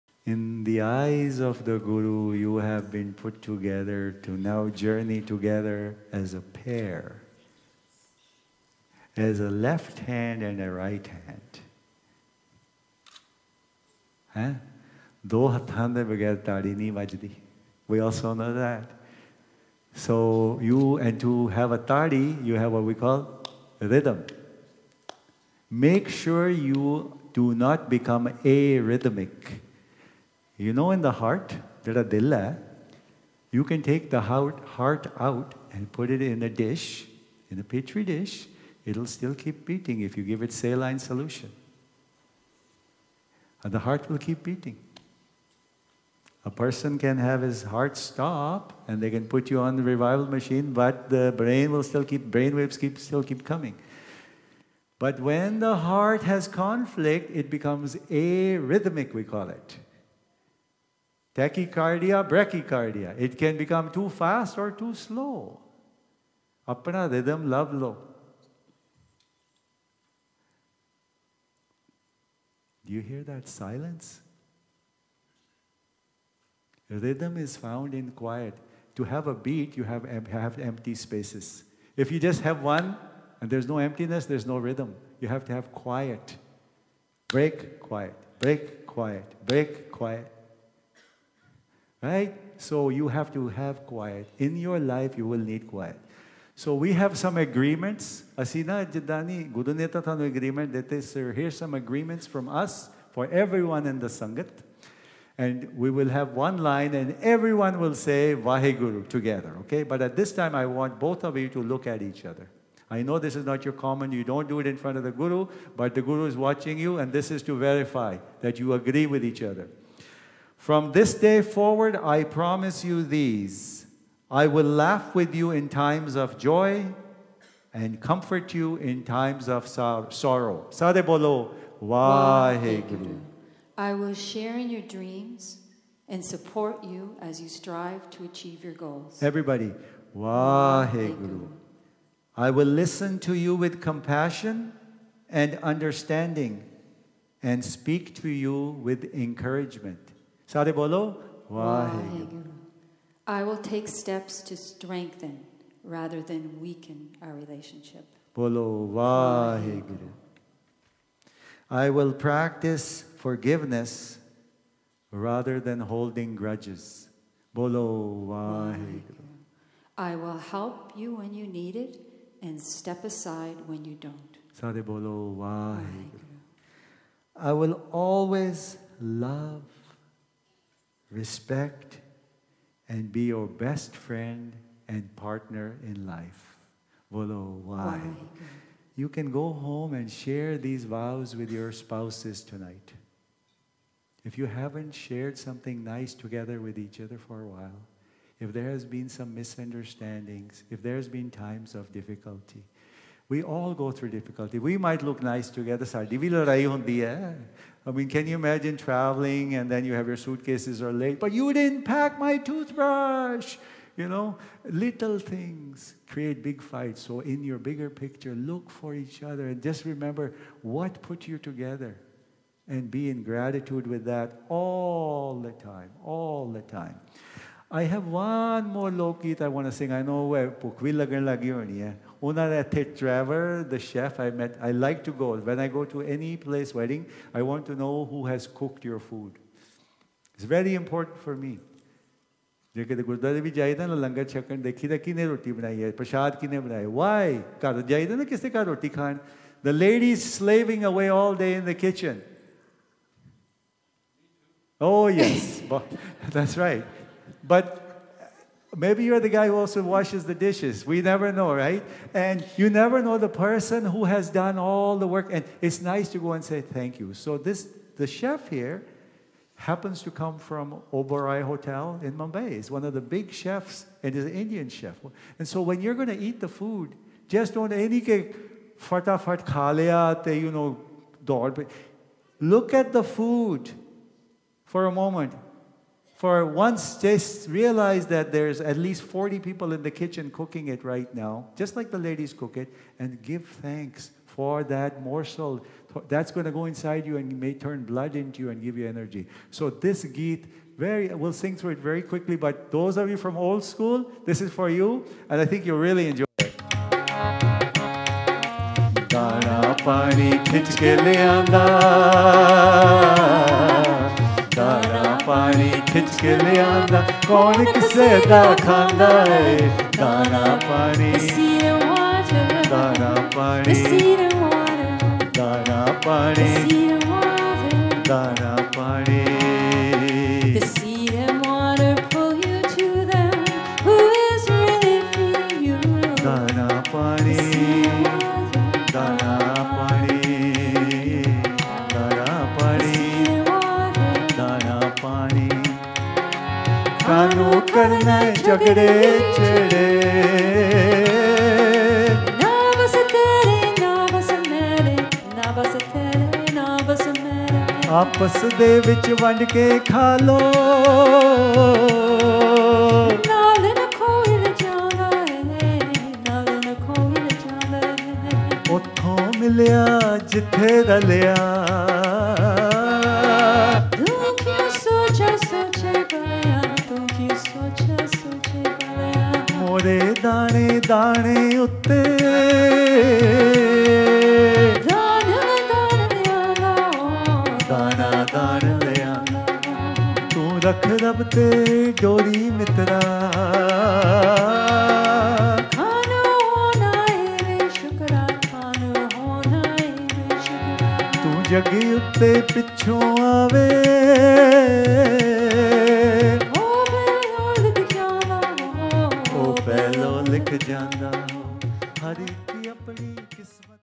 Sikh Destination Wedding Toronto Hotel | End of Ceremony Pt 18
Sikh Destination Wedding at a hotel in Toronto.  See the last of the Sikh Ceremony.